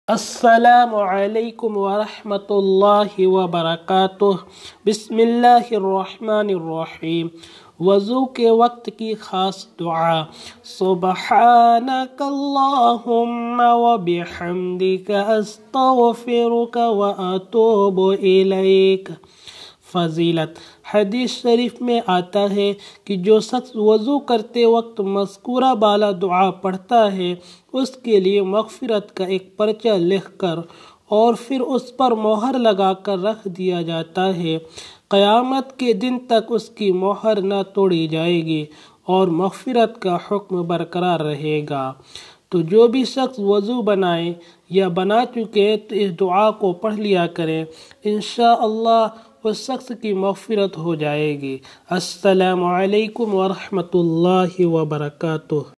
Dua For Abulation - Waju K Waqt Ki Khas Dua - Maghfirat Ki Dua - Daily Dua For Every Muslim
Assalamu Alaikum Warahmatullahi Wabarakatuhu

Dosto

Aaj Ki Is Video Me Dua For Abulation. Waju K Waqt Ki Khas Dua Ko Bataya Gaya Hai. Maghfirat Ki Dua Ko Bataya Gaya Hai. Daily Dua For Every Muslim.